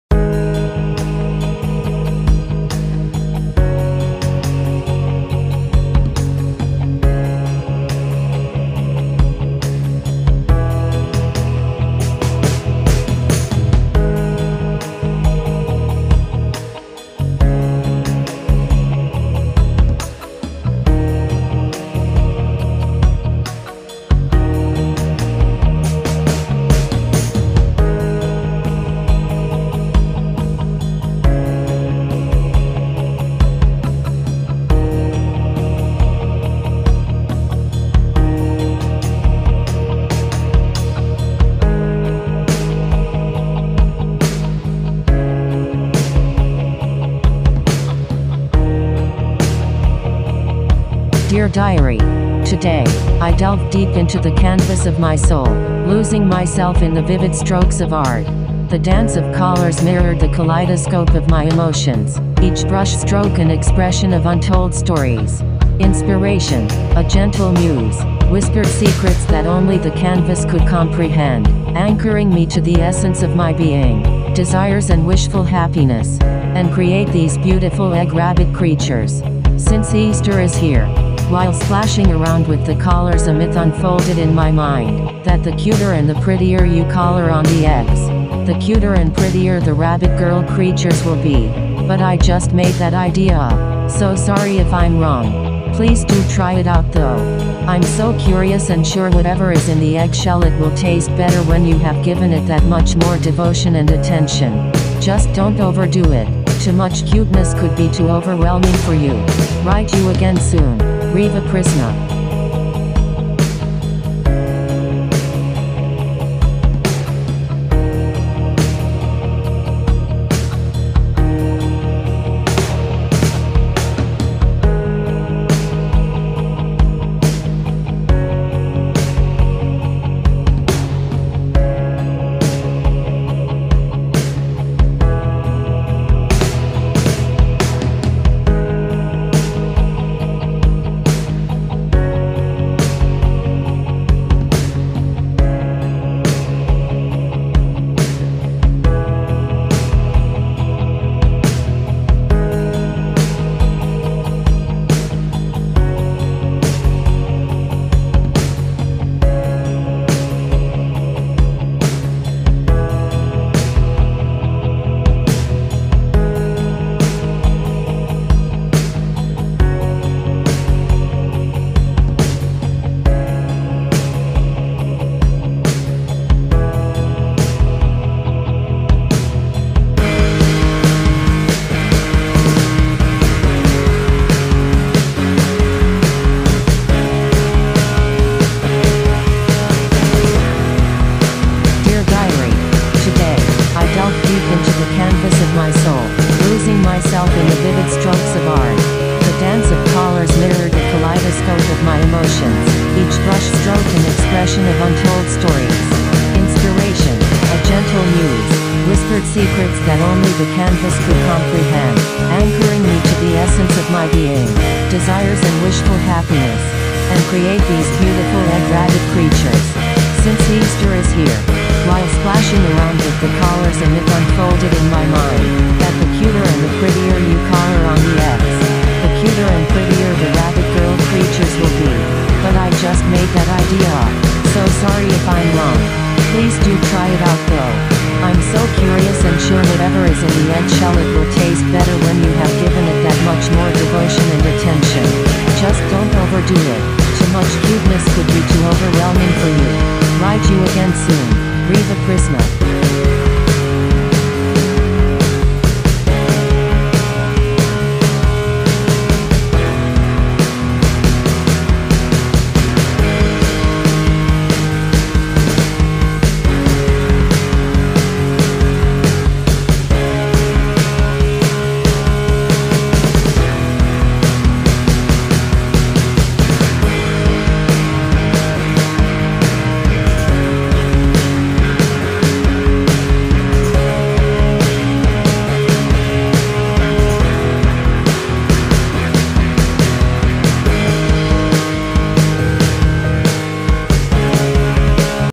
(S3E15) “Hi!“ using procreate, Full length paint Timelapse
!!Warning!! there can be some screen flashing!

This weeks diary entry by Reva Prisma.

“Monkey see, monkey do” - learning
Watch me draw so you can get better in drawing. Learn from the mistakes you see me make. 😘 Tell me about it so I can learn with you!

Art programma; Procreate
Device; iPad Pro (12.9-inch) Wi-Fi 256GB (3rd generation)
Styles; Apple Pencil (2th generation)

Canvas; 40x40cm (16”x16”) 300dpi (4724x4724px)
Colour profile; RGB (Display P3)
Saved as .PNG

for sketching; a normal BiC 0,7mm HB#2 in my real life Moleskine sketchbook.

Brushes most often use (all are native to procreate);
for sketching in procreate - 6B pencil
For line-art; Inking - Syrup
For textures and colour shading; Airbrushing - Soft Inking - Syrup
For lowlights & highlights; Inking - Syrup

I mostly toy with the transparency of layers that I clip masked to give texture and light effects. Not with layer effects in this piece.

I will try to answer as good as I can.

Video edited with; CapCut
Design elements made by myself in procreate.